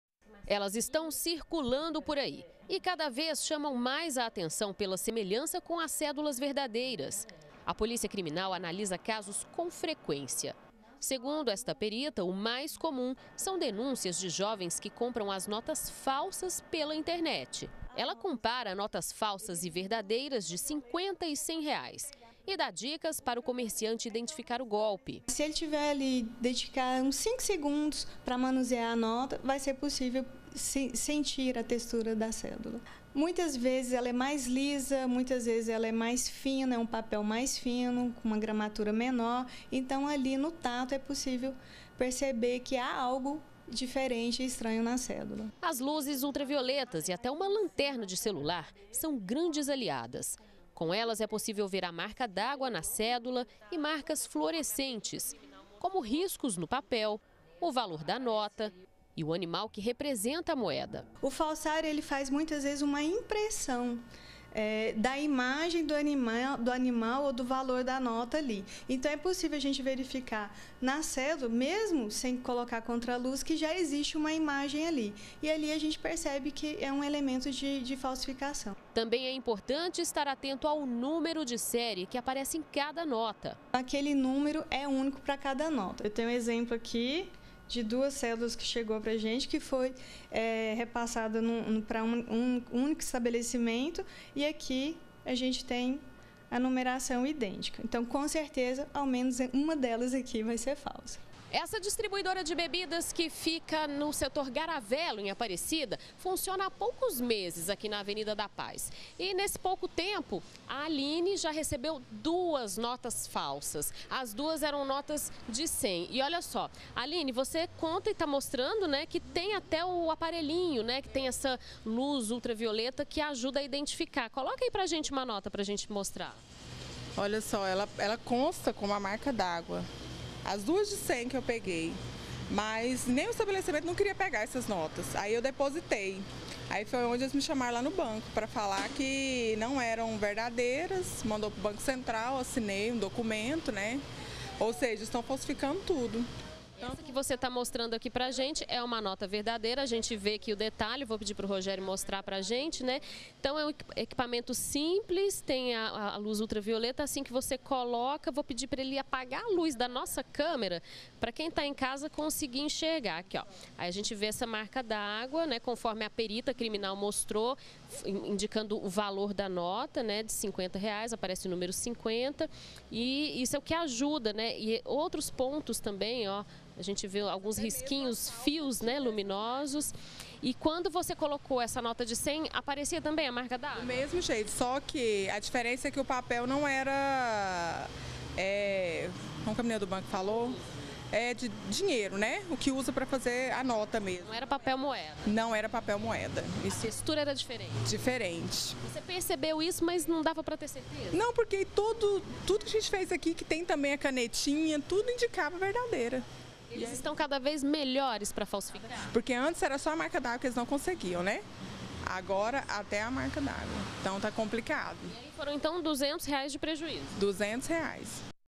BG - Notas falsas no comércio de Aparecida - 17-10-2017
Comerciantes em aparecida de Goiânia estão ficando no prejuízo pela quantidade de notas falsas recebidas. Donos de supermercado, restaurante e açougue já foram vítimas várias vezes.